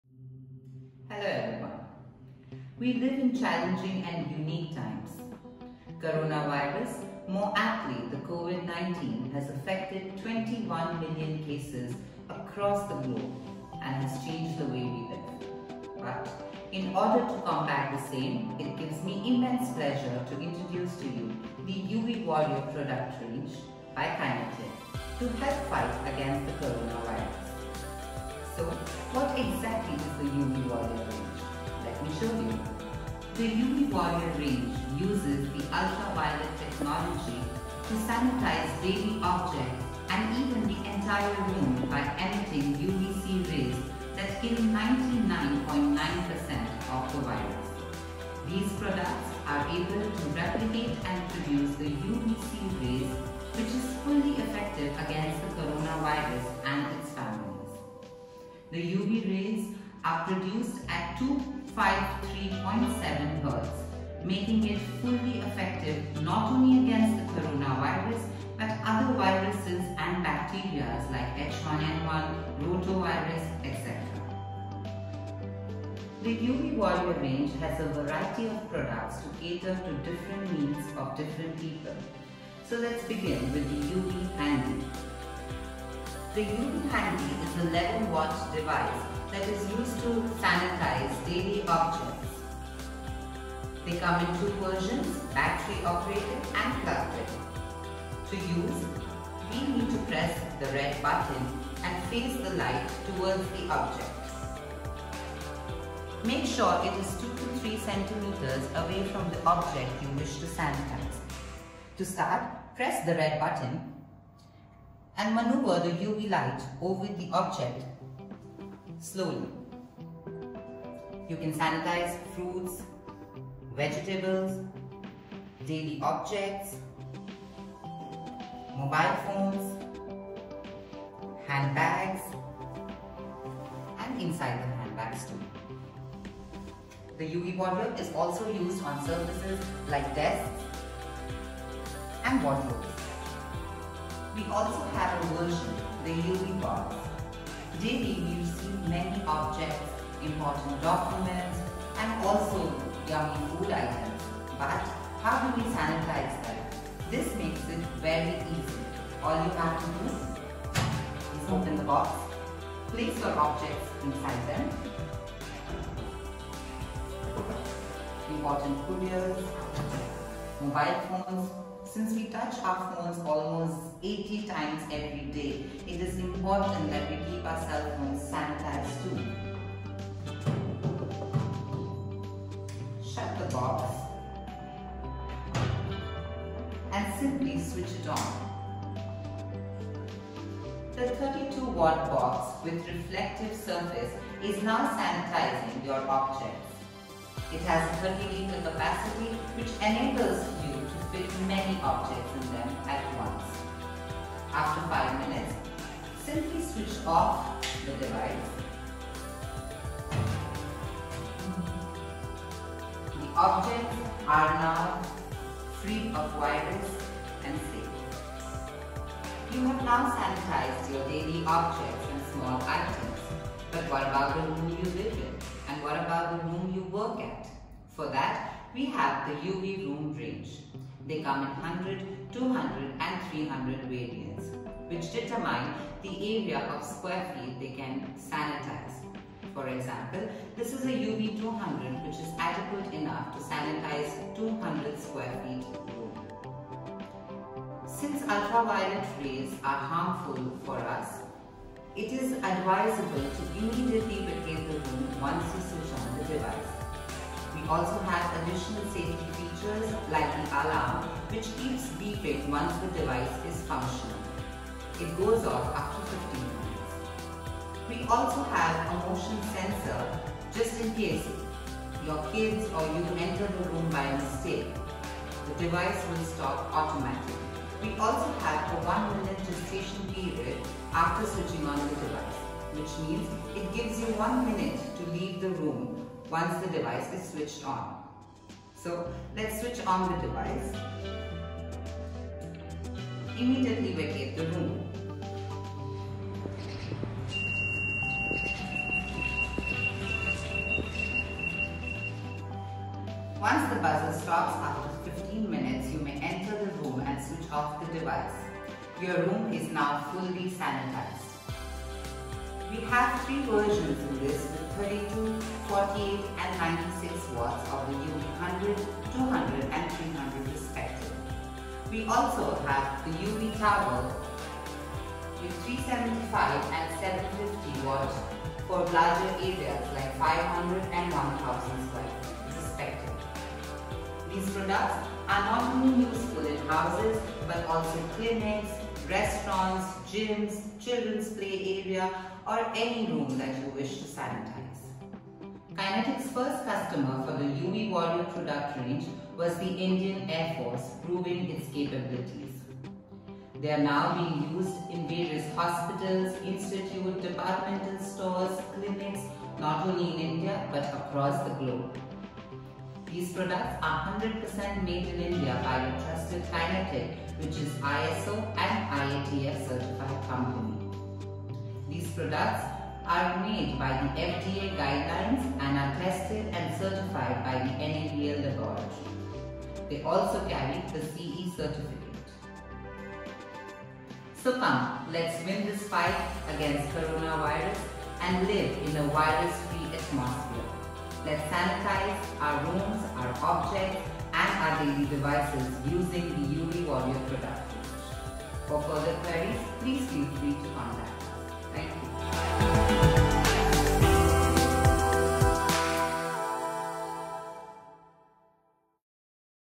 Kinetic UV Warrior Product Range
The Complete description of UV Warrior range by kinetic. Products include: UV handy, UV box, UV room and UV tower - to make every object and space completely free of any virus including the coronavirus. Demo and description by Arzoo Alamin.